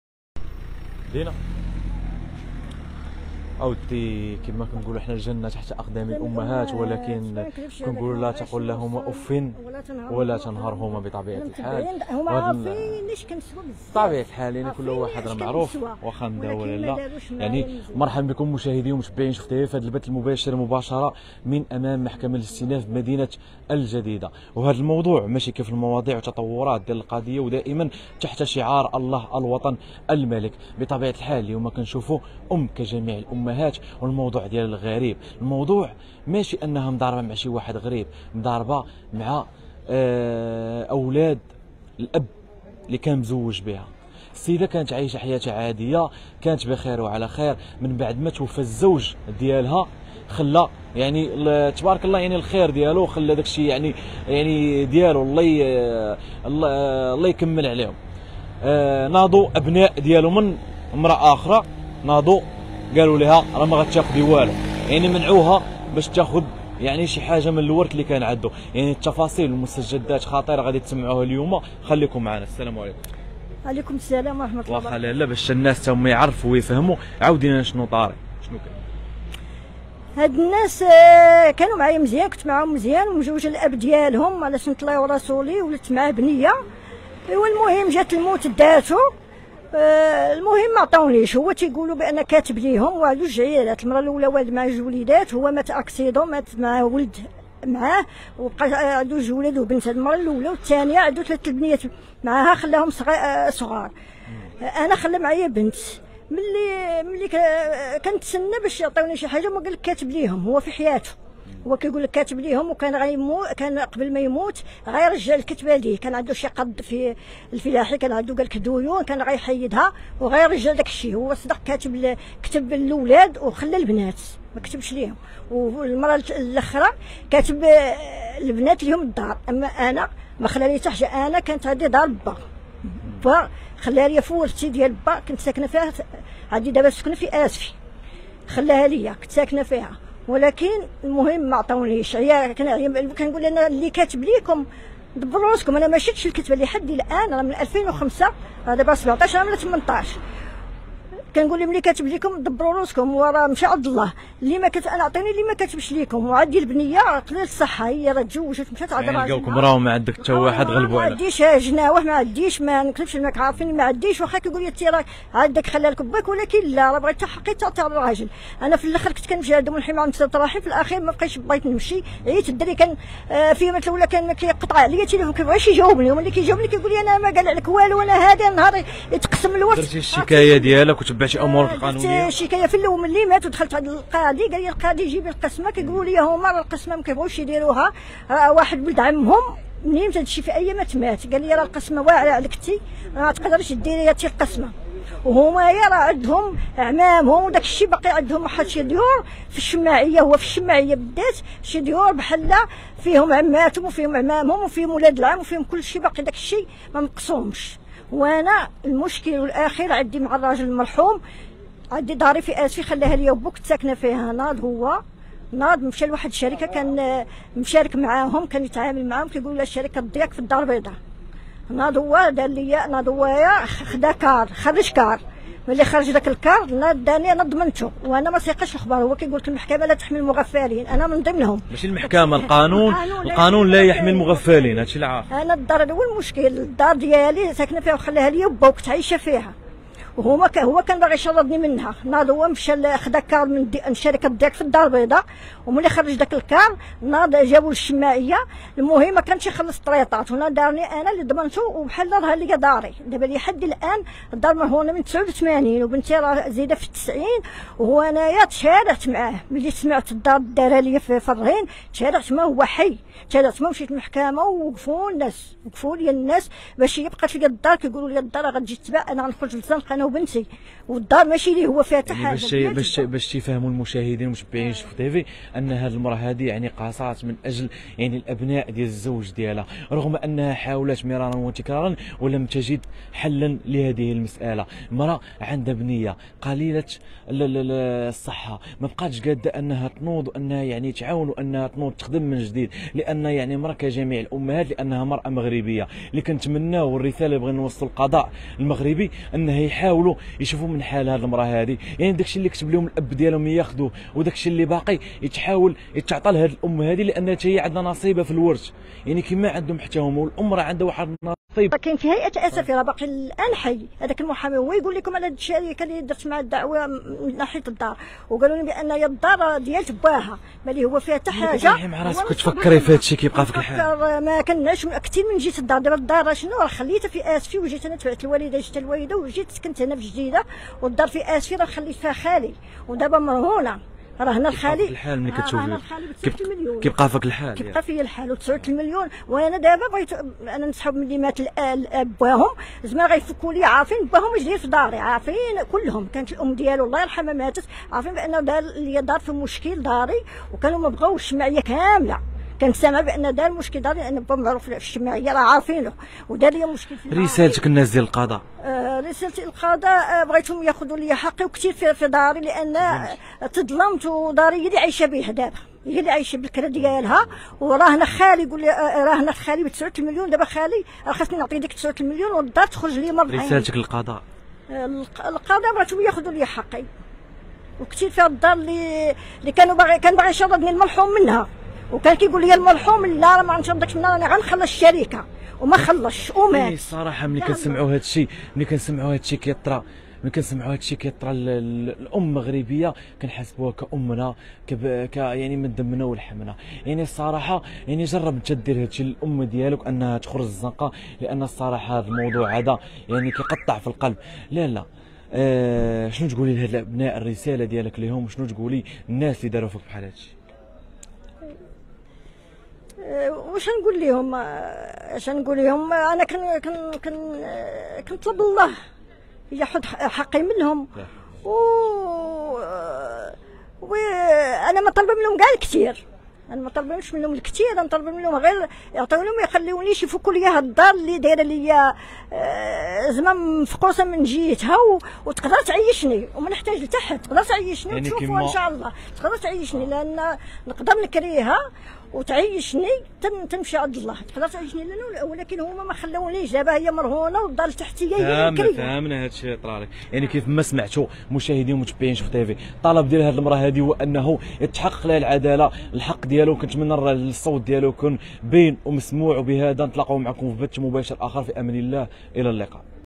من الجديدة: أرملة فحالة هستيرية بسبب ولاد راجلها لي مات ولي بغاو ياخدو ليها الورث ويخليوها بلا والو
مباشرة من الجديدة: أرملة فحالة هستيرية بسبب ولاد راجلها لي مات ولي بغاو ياخدو ليها الورث ويخليوها بلا والو